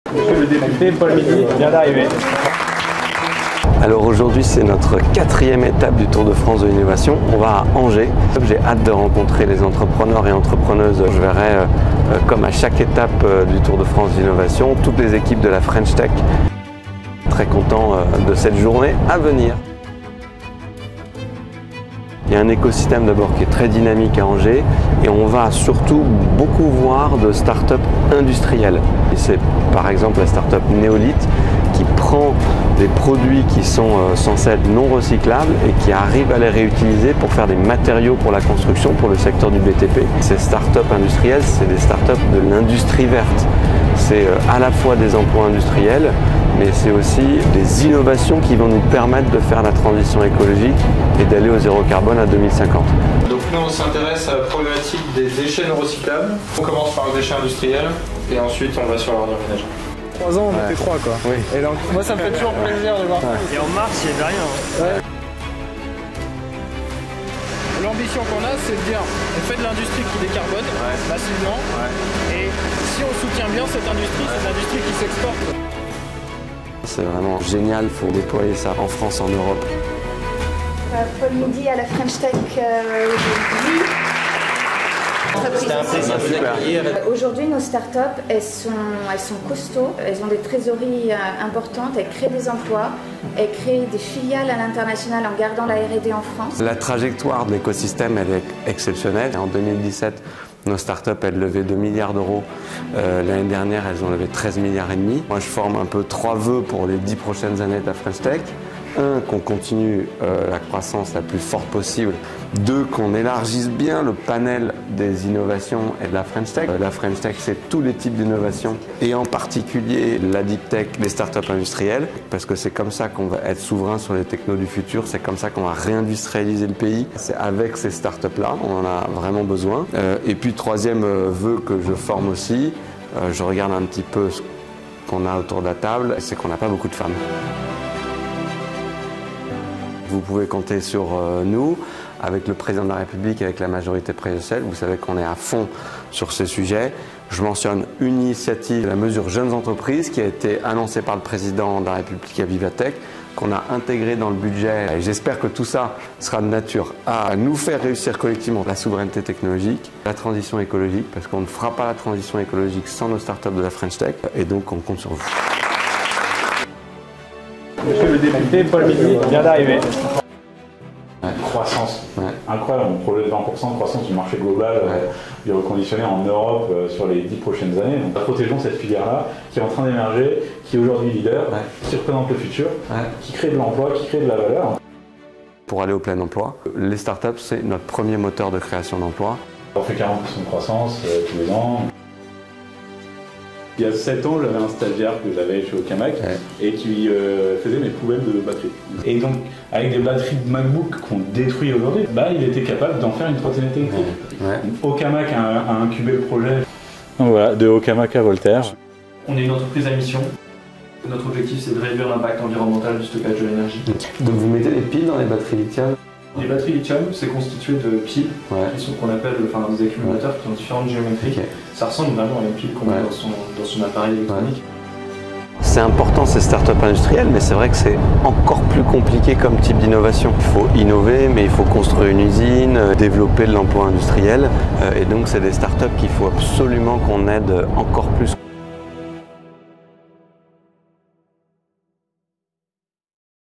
Start-ups industrielles : une journée dans l'écosystème à Angers
Start-ups industrielles et DeepTech : l’innovation au service de l’écologie ♻️

Dans le cadre de mon Tour de France de l’Innovation je suis allé à la rencontre de l’écosystème de start-ups à Angers !

Bravo à la French Tech Angers et à tous les entrepreneurs qui innovent dans le digital et la DeepTech, c’est aussi avec eux que nous ferons la transition écologique 👏

👉 Soutenir nos start-ups c’est soutenir la création d’emploi, la souveraineté technologique et la réindustrialisation de notre pays 🇫🇷